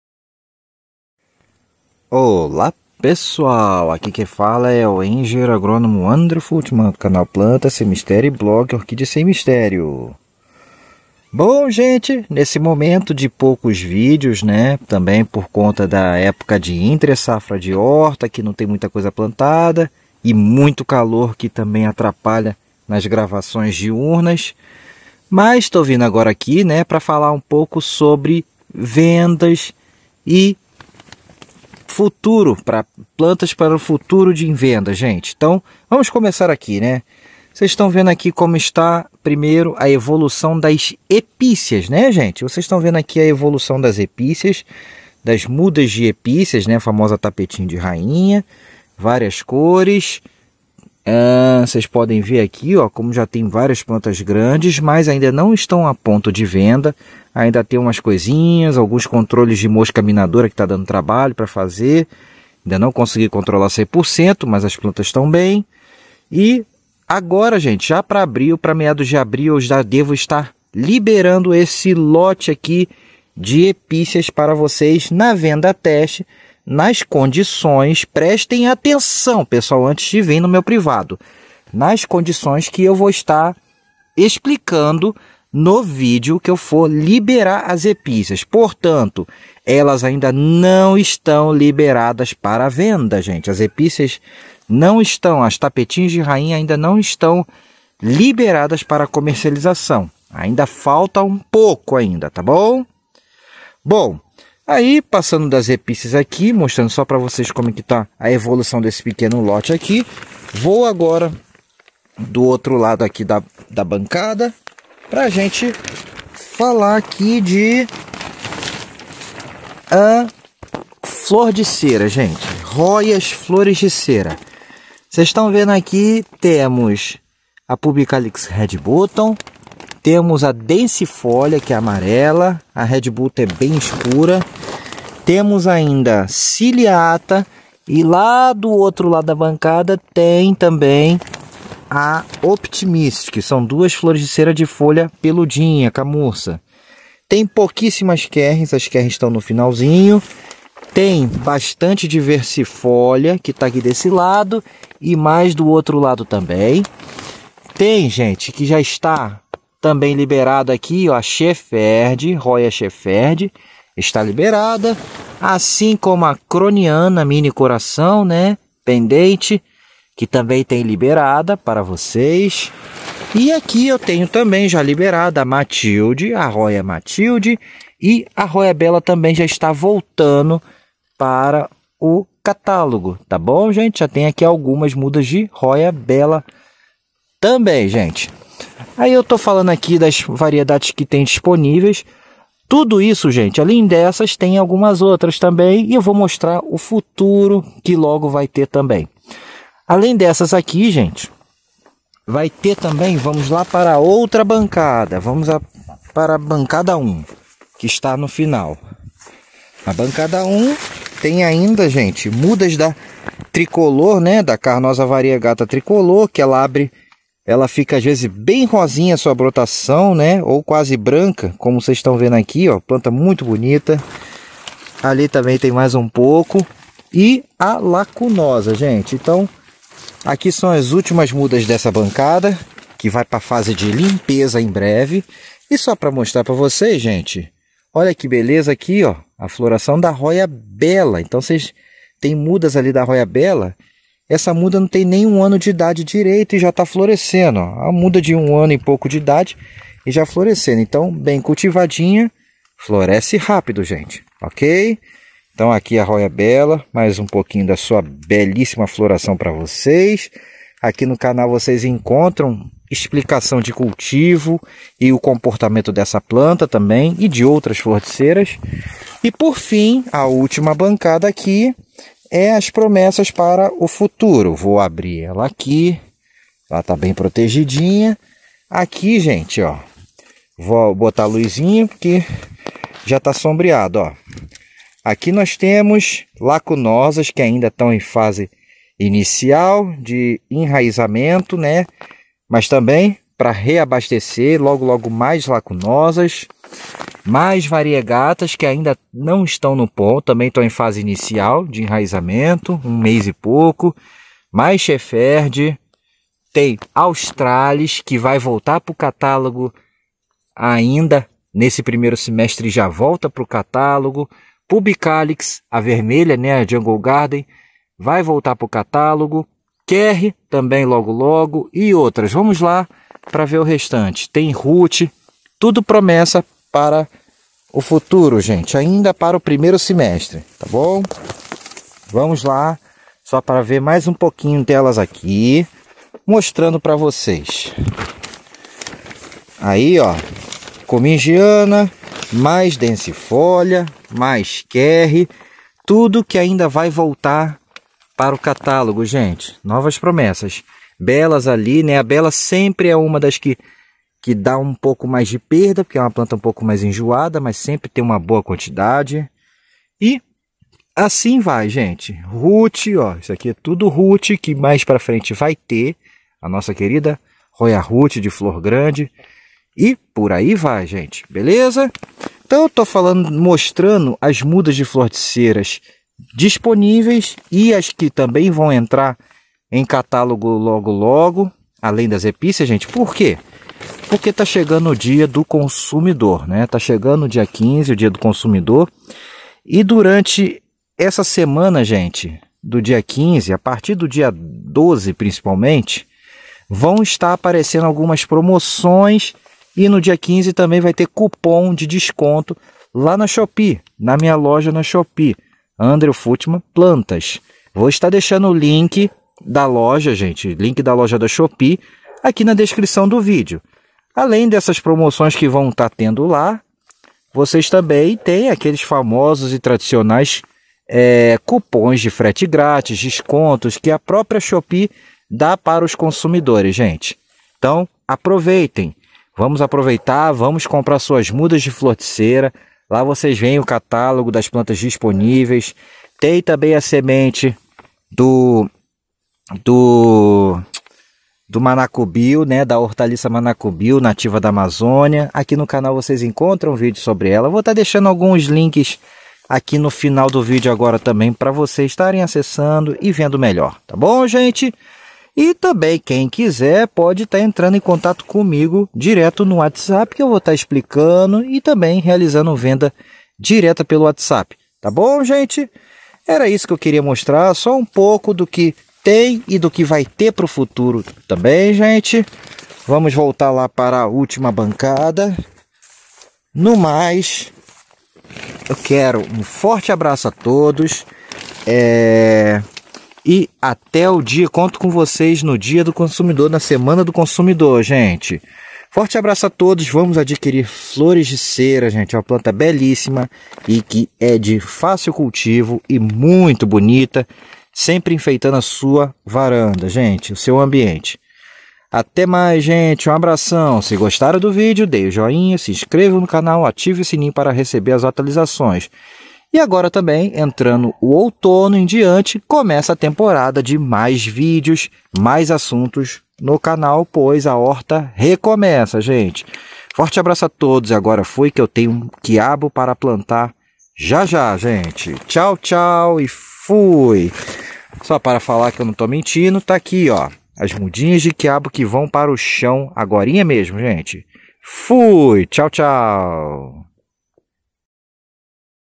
Dia Do Consumidor Chegando! Confiram O Que tenho Disponível e O Que Terei em Breve!
Este vídeo é para divulgar as plantas que estão disponíveis em breve e as que já estão disponíveis, além de alertar para as promoções que estarei realizando no dia do consumidor, 15 de março. Confiram!!